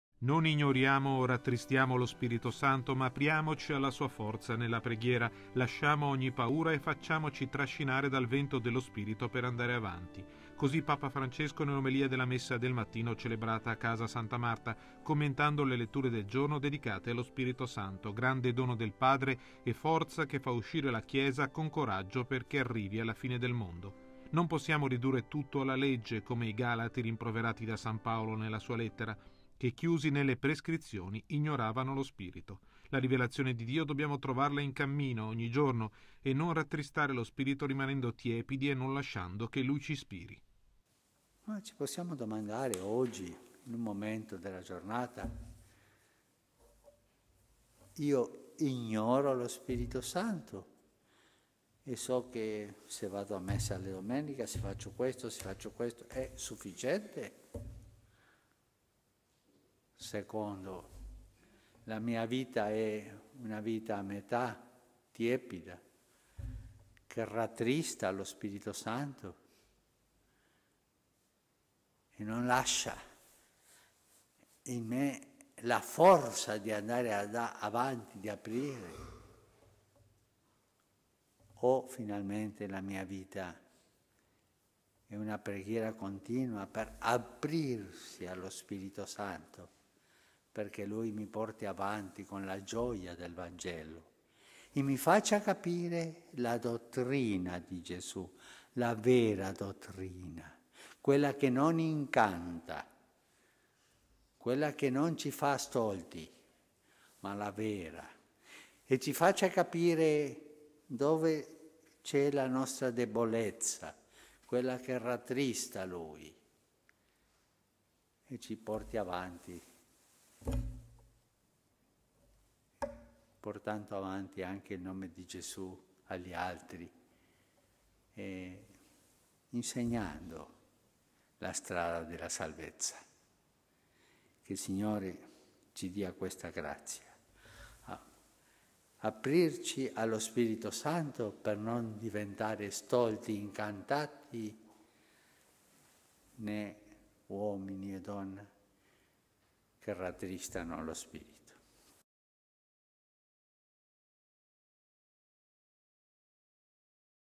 Messa Santa Marta, 06-10-2016
Apriamoci allo Spirito, il solo che fa andare avanti

Papa Francesco, nell’omelia della messa del mattino a Casa Santa Marta, ha sottolineato che la vera dottrina non è rigido attaccamento alla Legge, ma è la rivelazione di Dio che si lascia trovare ogni giorno di più da quanti sono aperti allo Spirito Santo.